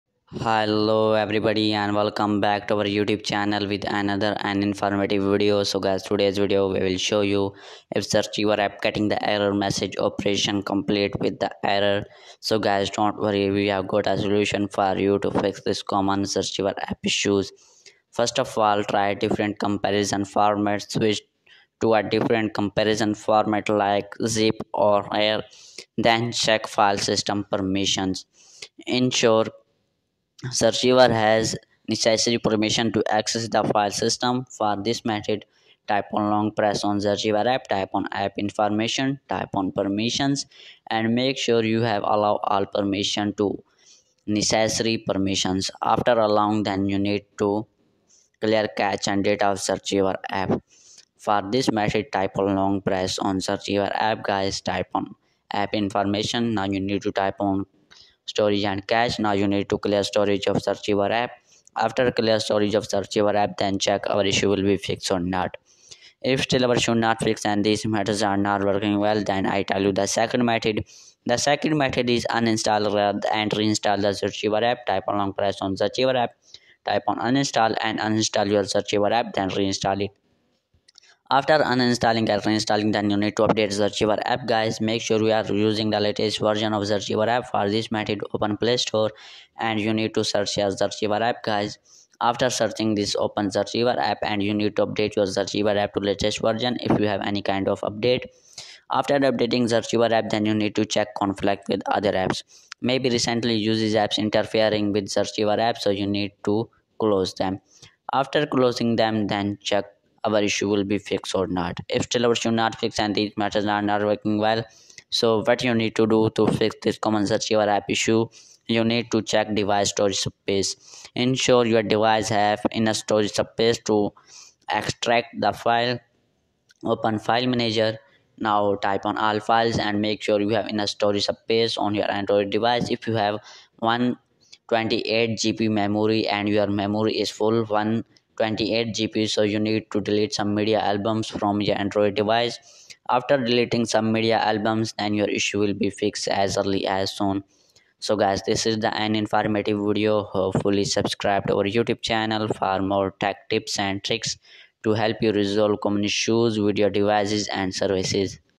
How To Fix ZArchiver Operation Completed With Errors
Here's How To Fix ZArchiver Operation Completed With Errors.

If ZArchiver Getting The Error Message?Operation Completed With Errors. Don't Worry We’ve Got A Solutions For You. In This Video We’ll Show You How To Fix This Common ZArchiver Issues.